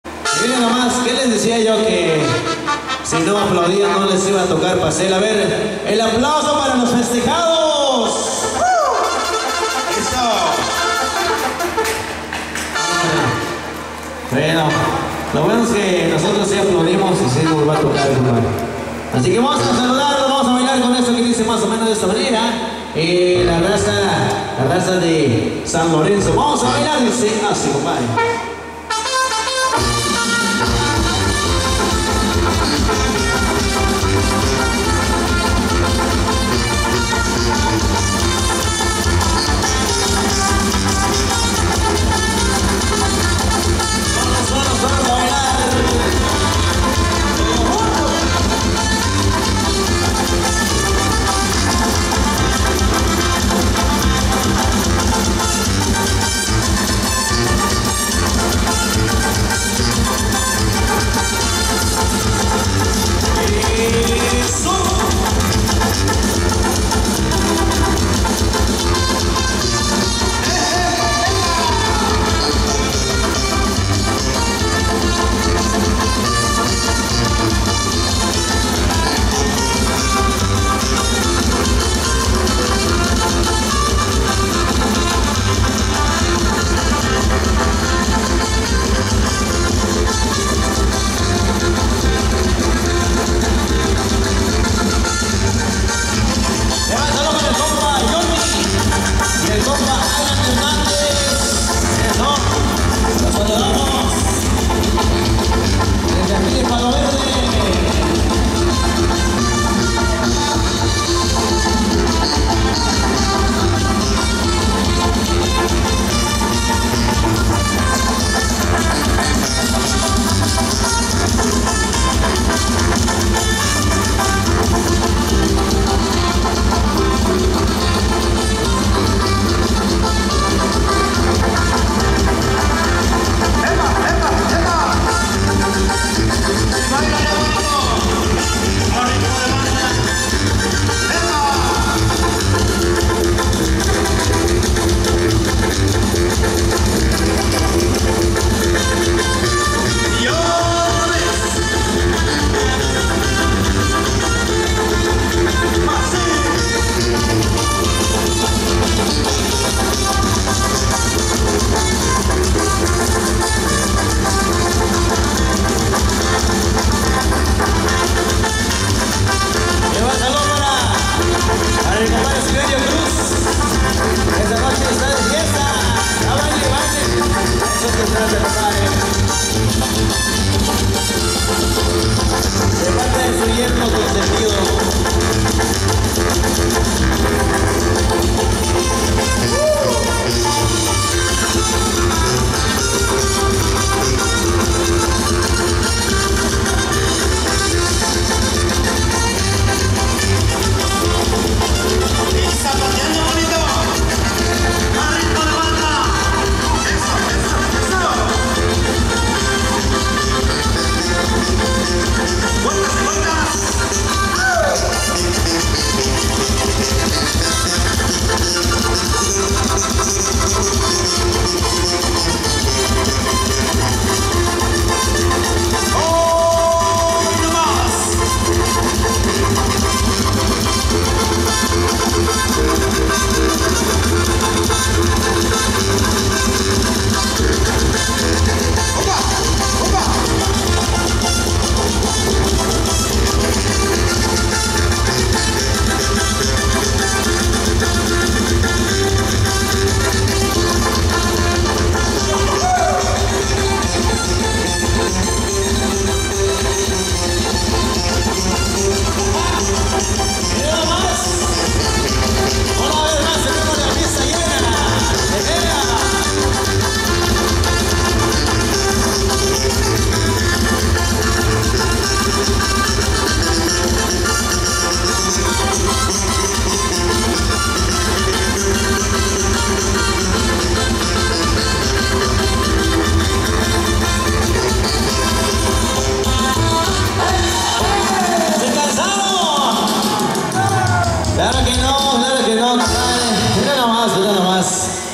Zonte Musical,Banda Electronica, Palo Verde Huehuetla Hgo, 27-05-2024.
DESDE TIERRA OTOMI  EL GRUPO ZONTE MUSICAL AL RITMO DE BANDA, PALO VERDE HUEHUETLA HGO, GRAVADO EL 27 DE MAYO DEL 2024.